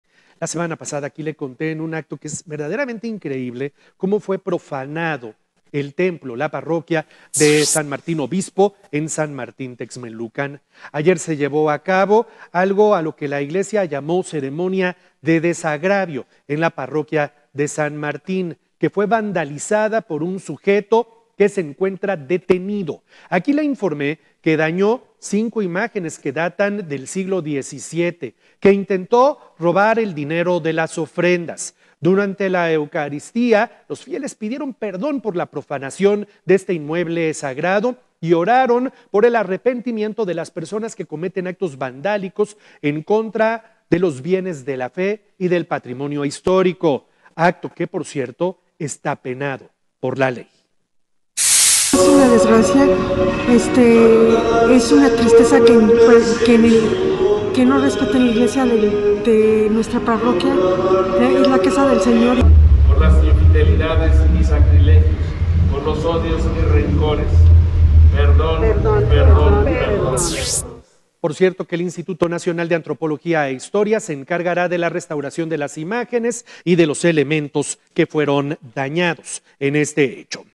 Ceremonia de desagravio || Noticias con Juan Carlos Valerio
Se realizó una ceremonia de desagravio en la parroquia de San Martín Obispo en Texmelucan que fue vandalizada por un sujeto el cual se encuentra detenido.